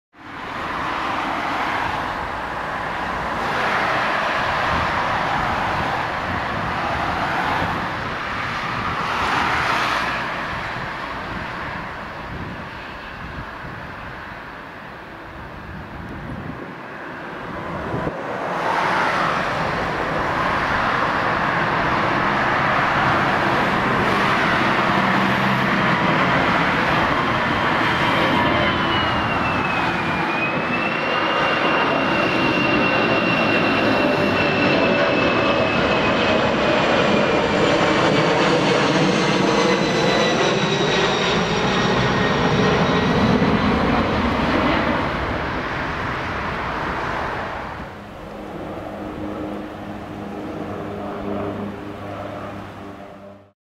A320 Sharklets Lufthansa D-AIZX go around at Hamburg Airport
First flight and go around runway 15 at Hamburg Airport.
New A320 Sharklets D-AIZX for Lufthansa. This is LH's Sharklet Nr. 9